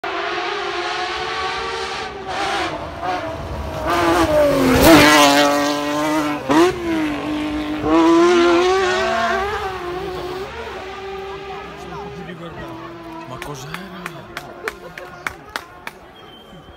EPIC JUMP Sipsz-Bregoli in Radicofani LANCIA STRATOS pure SOUND - RALLY VAL D’ORCIA 2020
Epic Jump!

RALLY VAL D’ORCIA

LANCIA STRATOS

SUPER SOUND